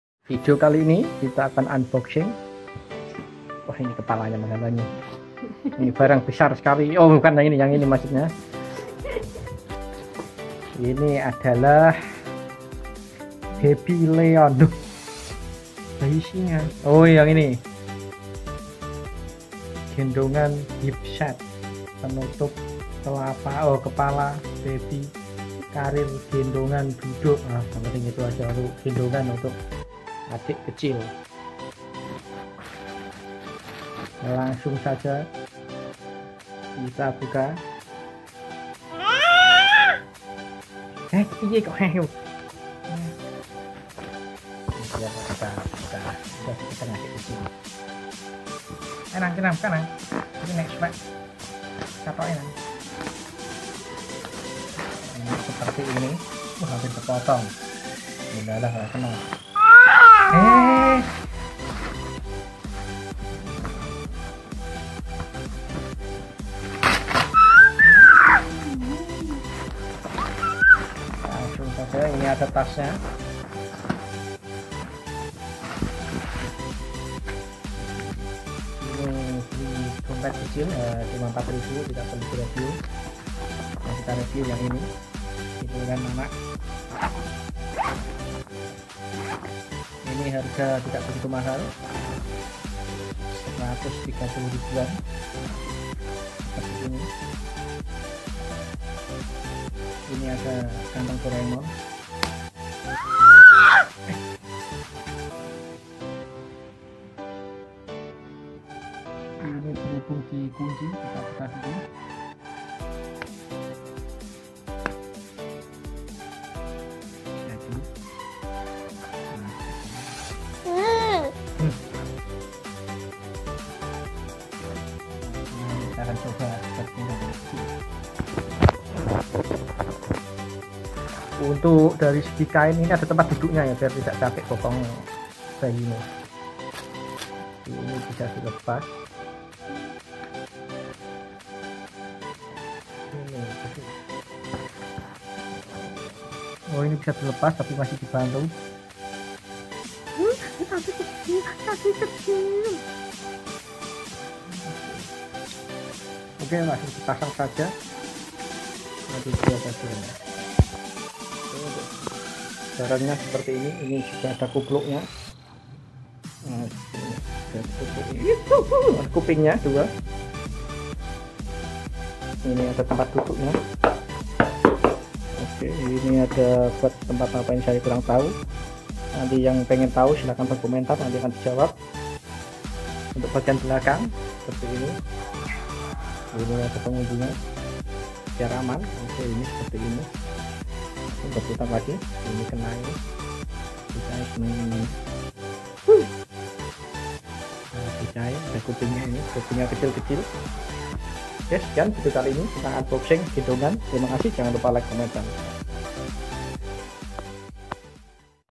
Unboxing Gendongan bayi Gendongan Hipseat + Penutup Kepala Baby
* Best Quality BABY LEON
 * Gendongan Hipseat Multifungsi + Penutup Kepala
 * Bahan lembut dan halus nyaman untuk si kecil dan bunda
 * Desain khusus dengan pengaman yang kuat dan aman untuk si kecil dan bunda
 * Hipseat dan Strap bisa dilepas / dipasangsesuai kebutuhan
 * Terdapat safety buckle untuk melepas dan memasang hipseat
 * Hip SEAT Model dengan BANTALAN support anti capek
 * Multi posisi depan belakang samping 360 derajat, Bahan COTTON & kuat, bisa di
   cuci
 * 0-3 th / maximal 20 kg
 * Bisa untuk Lingkar Pinnggang Mommby BB 110Kg
 * Bantalan HIPSET untuk menyusui/gendong simple
 * Multi pengunci sabuk,Breathtable material & menyerap keringat,Cocok untuk
   treveling/jalan ke mall